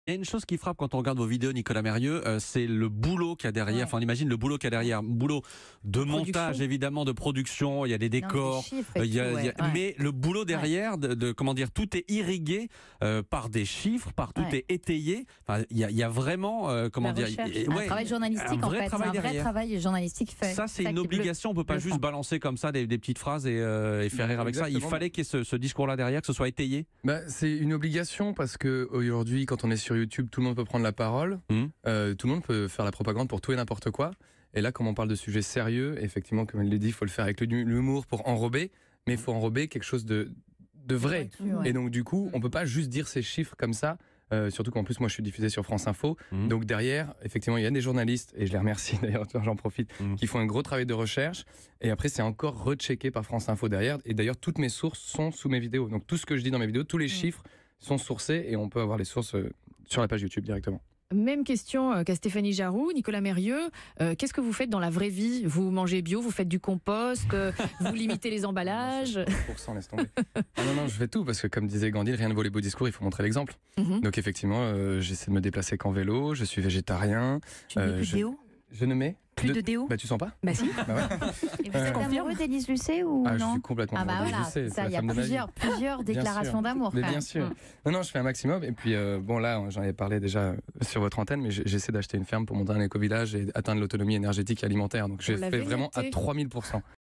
Rire sur l'écologie : pour Nicolas Meyrieux, c'est possible, à condition "de ne pas sortir des ch…
Rire sur l'écologie : pour Nicolas Meyrieux, c'est possible, à condition "de ne pas sortir des chiffres comme ça"